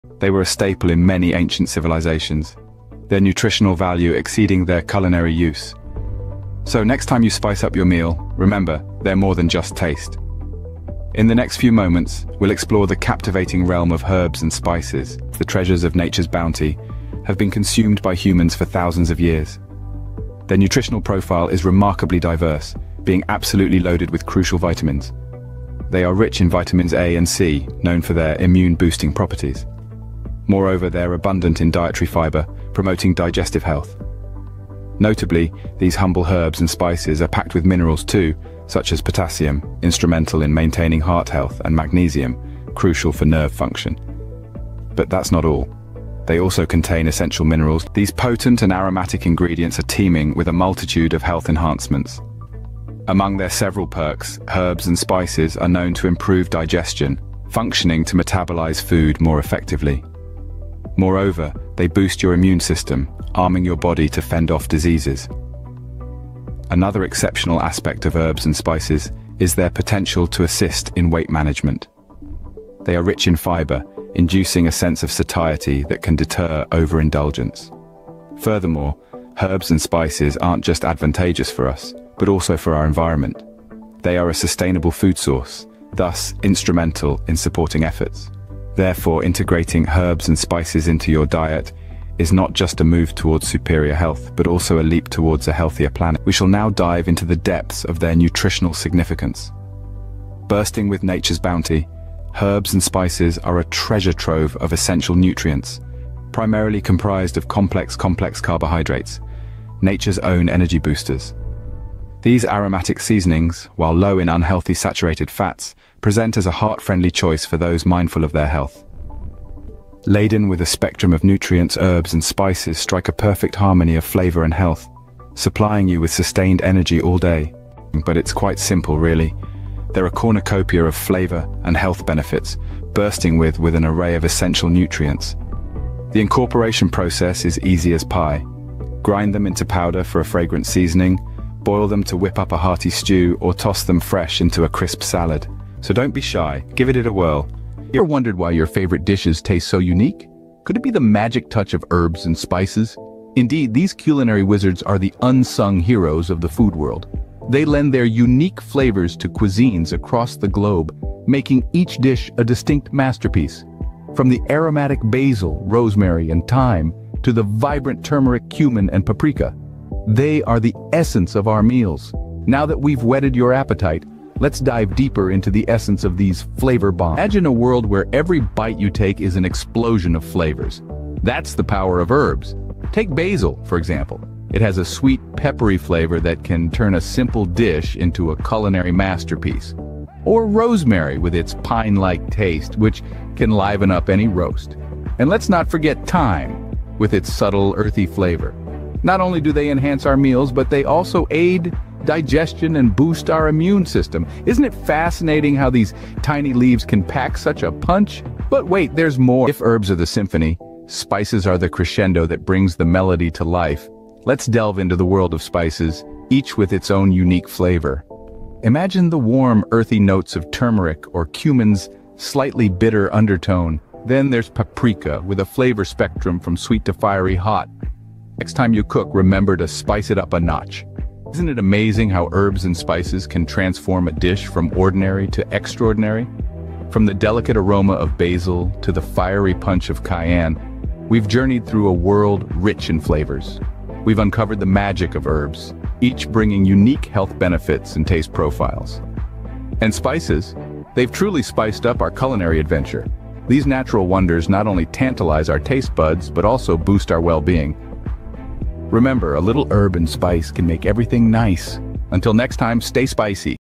Unlocking Taste & Health with Herbs & Spices! 🧂👨‍🍳👌
In this video, we dive into the fascinating realm of herbs and spices. Join us on a flavorful journey as we uncover their intriguing facts, explore their incredible health benefits, and learn how they can transform our meals from ordinary to extraordinary.

Did you know that herbs and spices have been used for centuries not only to enhance the taste of food but also for their medicinal properties? We'll reveal some surprising facts that will leave you amazed.

Whether you're a cooking enthusiast, a health-conscious individual, or simply curious about different cuisines, this video is for you! We'll discuss the essential herbs and spices from around the world and how their unique flavors can elevate any dish.

Get ready to tantalize your taste buds and improve your well-being with the power of herbs and spices. Together, let's unlock the secrets of these culinary wonders!